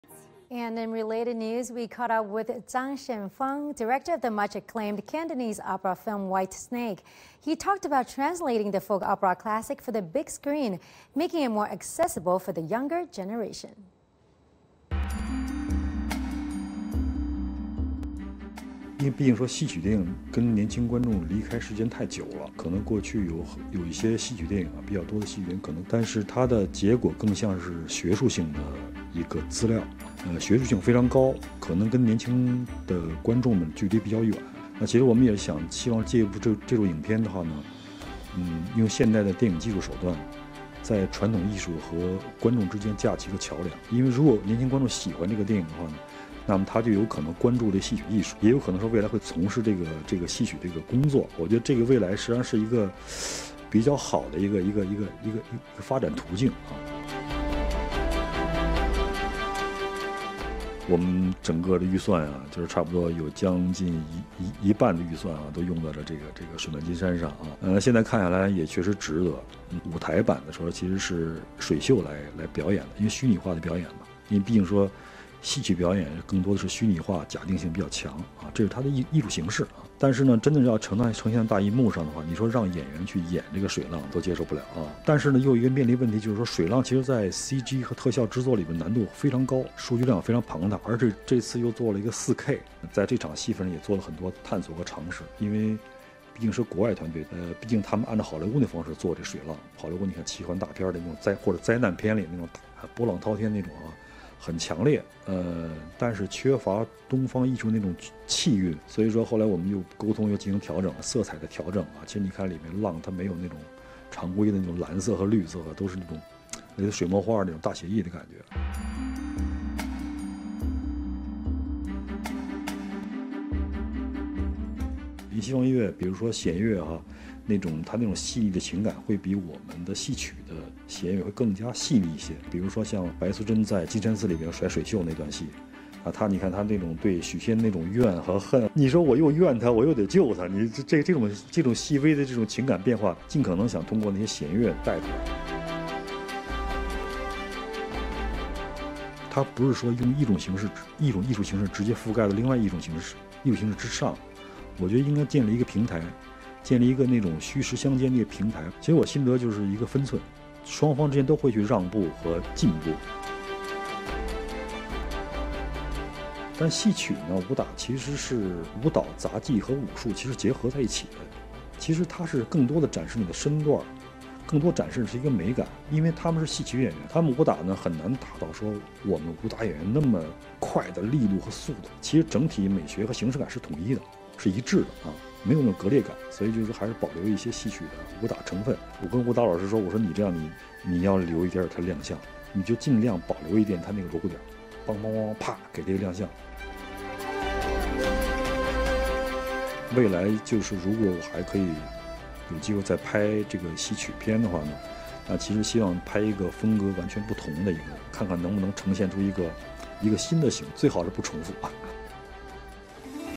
Director Zhang Xianfeng on Translating Folk Opera Classic for the Big Screen
WHITE SNAKE